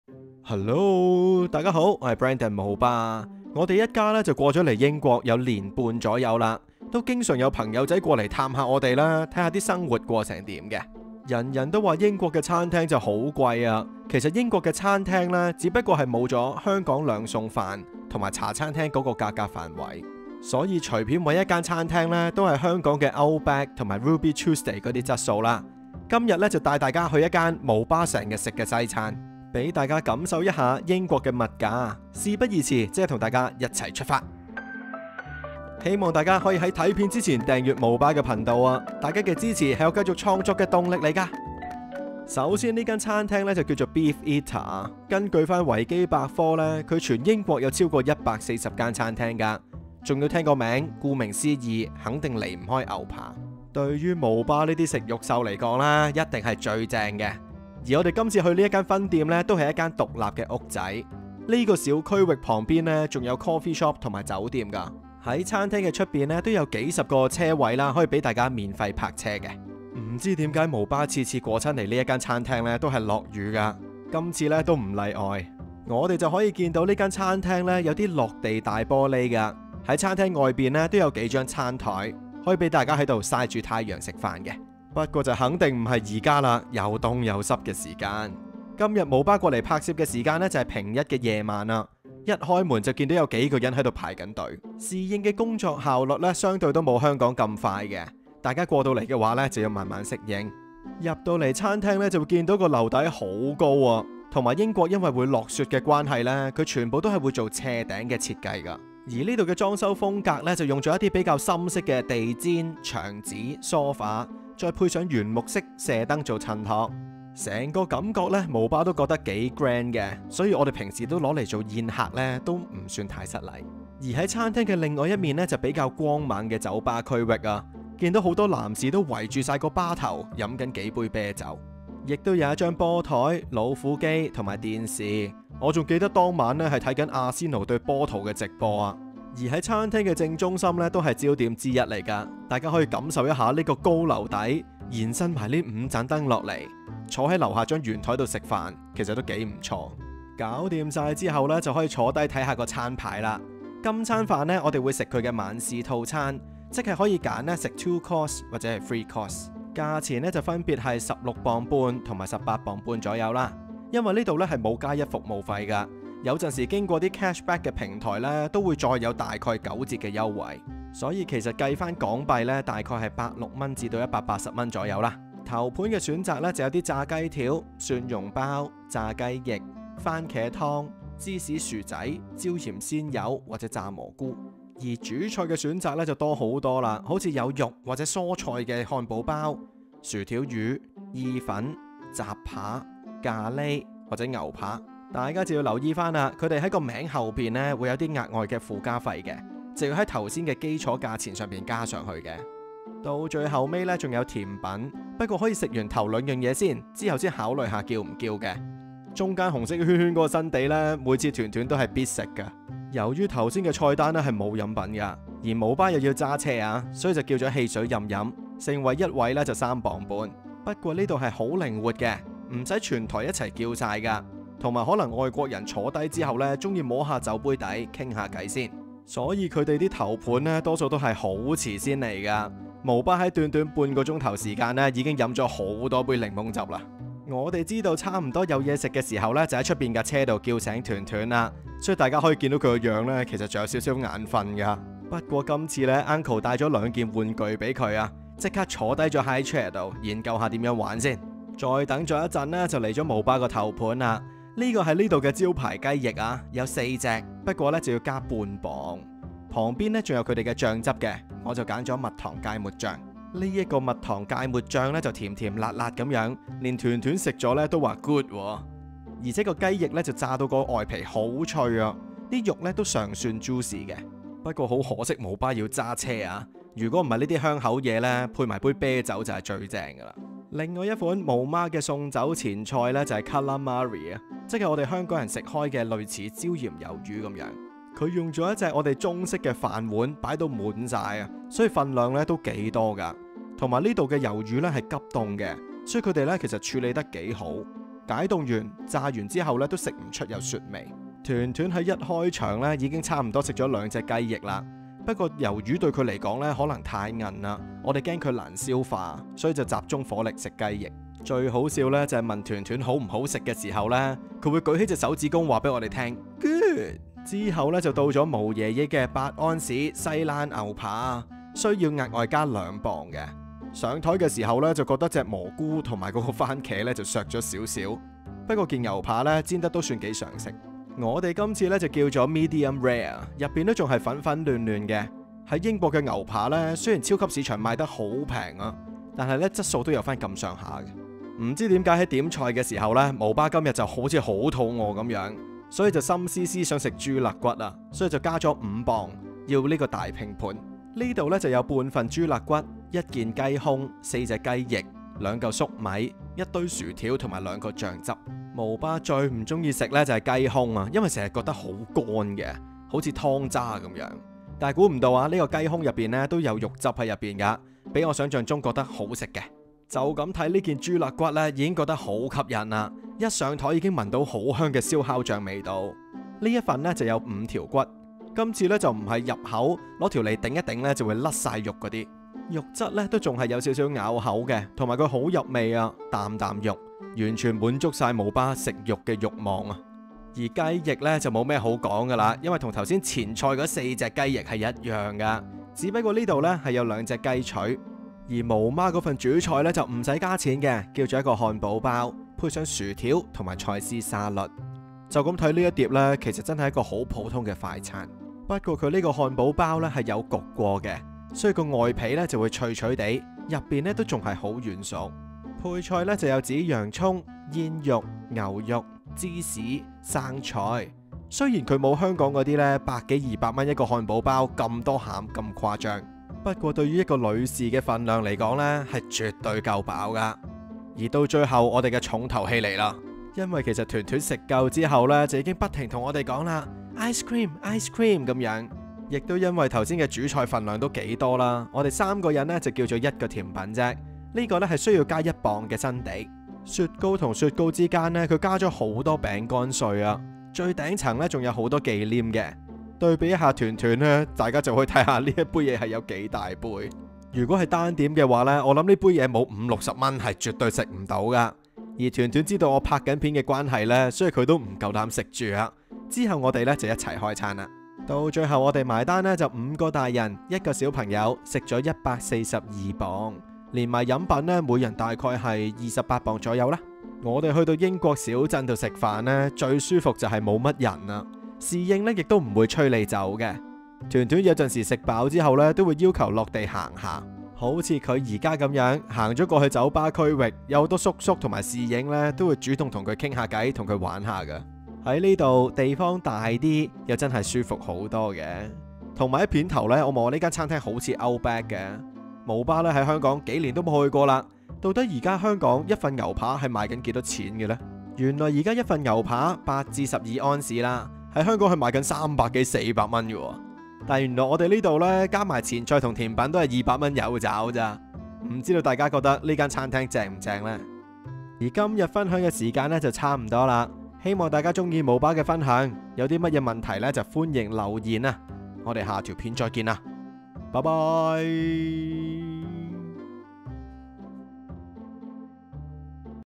《毛家回頭率最高既餐廳！》靚裝修！£18.45有齊頭盤 主菜 甜品！仲有酒吧區 直播足球賽事！@Beefeater 《🇬🇧毛爸英國生活小分享》
✨敬請留意✨
🔺此影片絕非廣告；

🙌🏻多謝大家支持及收看！

如果大家喜歡毛爸的影片，想支持毛爸/贊助毛毛玩具，萬分感激!

Directed and edited by Brendan毛爸.